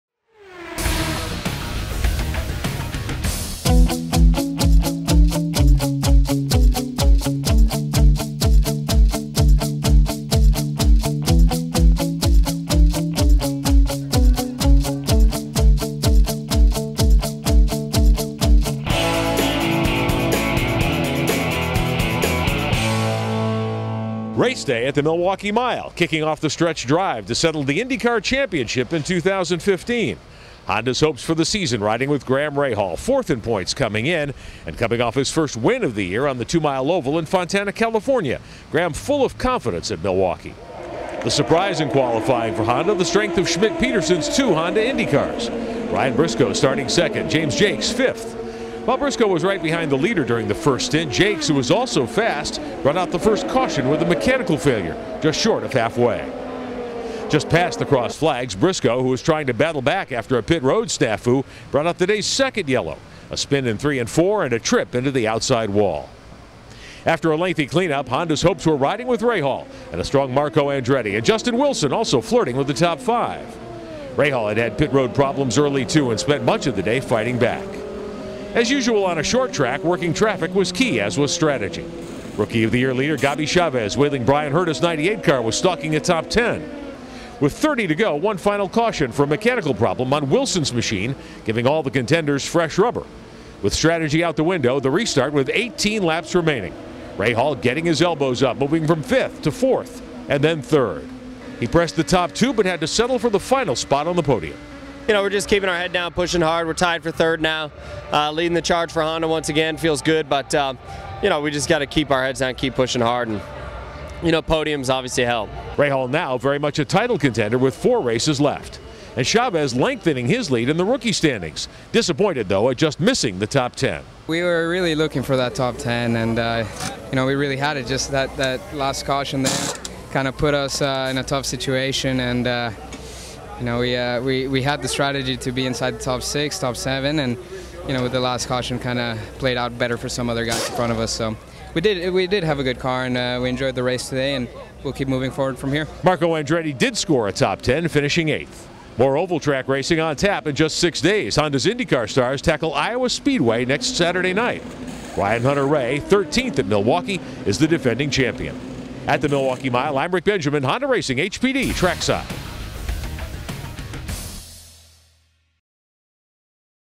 HPD Trackside -- ABC Supply Milwaukee Mile Indycar Race Report
It was another exciting IndyCar Series​ race at the ABC Supply Wisconsin 250​ at Milwaukee IndyFest​ with Rahal Letterman Lanigan Racing​ driver Graham Rahal​ taking his 5th podium of the season!